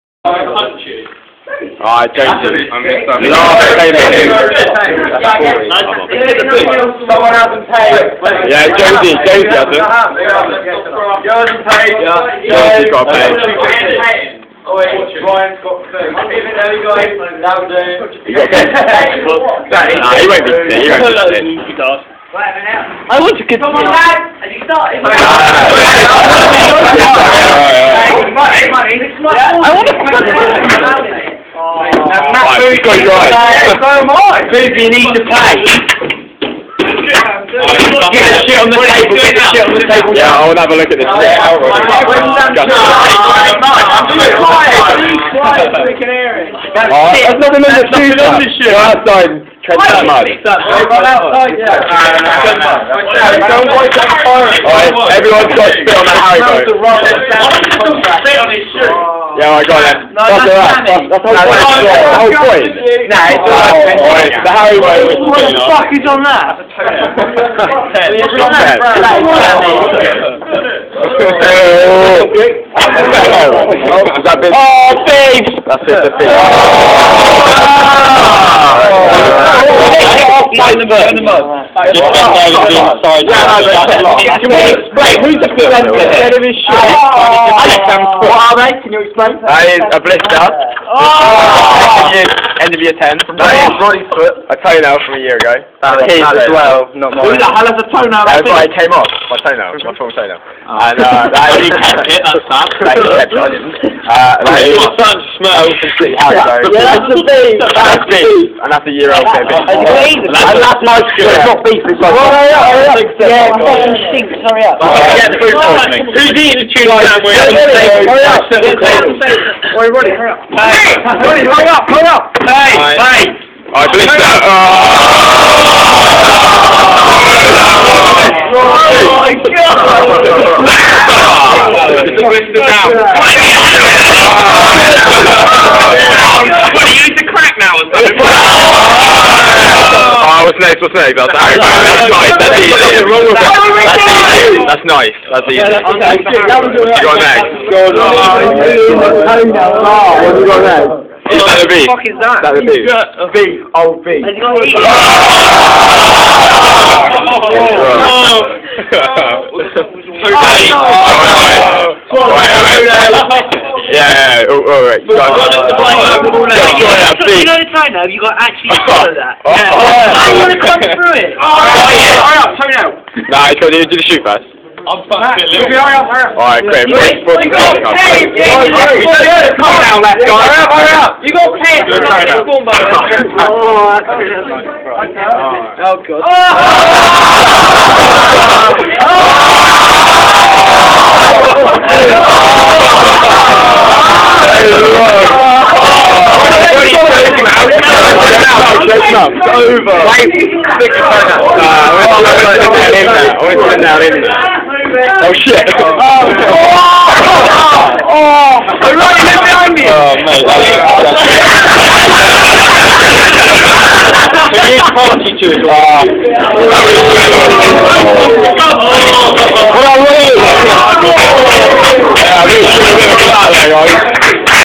Roddy's 5 disgusting deeds
Roddy Bailey eats
- a 6 month old blister
- a haribo ring covered in crap
- a year old piece of beef
- all sorts of the bottom of a shoe
- a year old complete toe nail